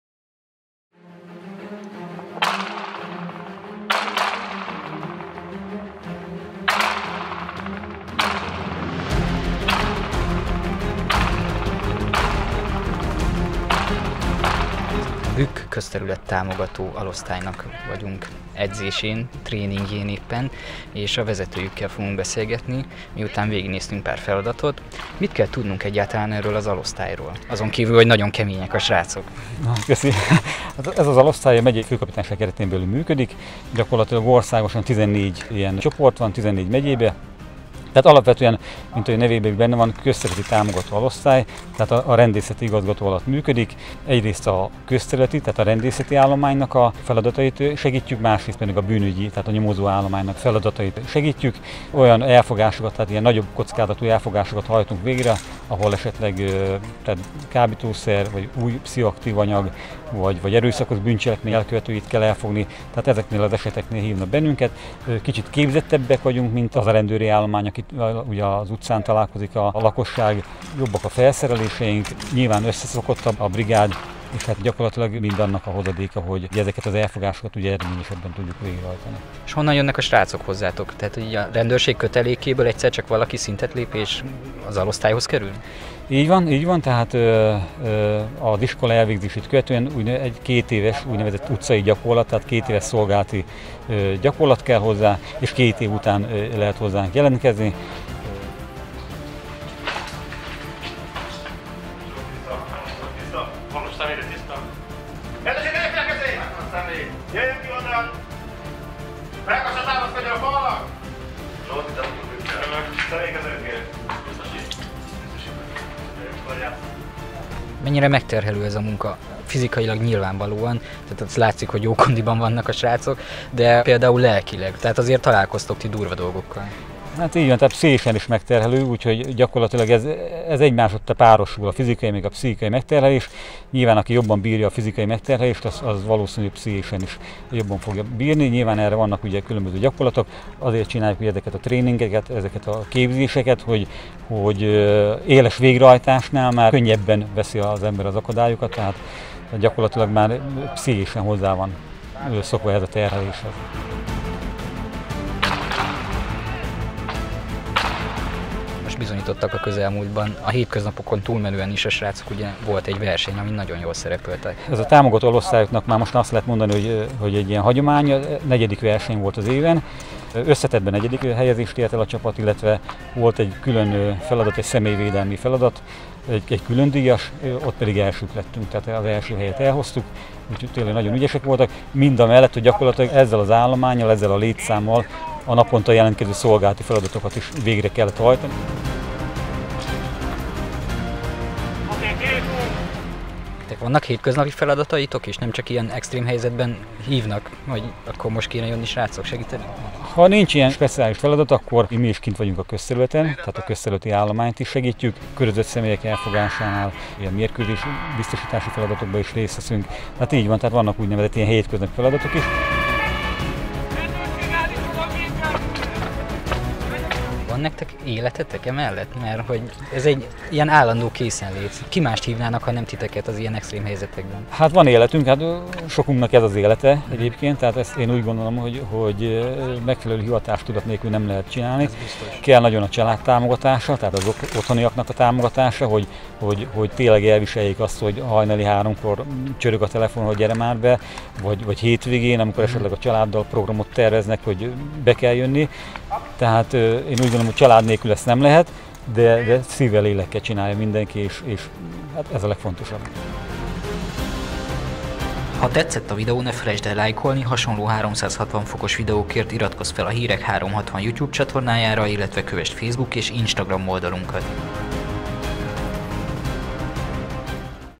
Ígérem jó leszek, csak ne bilincseljenek meg újra! - hirek360.hu
A Bükk Közterületi Támogató Alosztály a rendőrség kötelékében az a csoport, akiket mi hétköznapi emberek kommandósoknak hívunk. Ők ugyan szerények, és nem szeretik a kifejezést, de embert próbáló munka az övéké, ami ráadásul hatalmas elkötelezettséget is megkövetel. Gyakorlatozás közben látogattuk meg őket, ahol először kimenekítettek, majd megbilincseltek, de végig barátságosak és kedvesek voltak.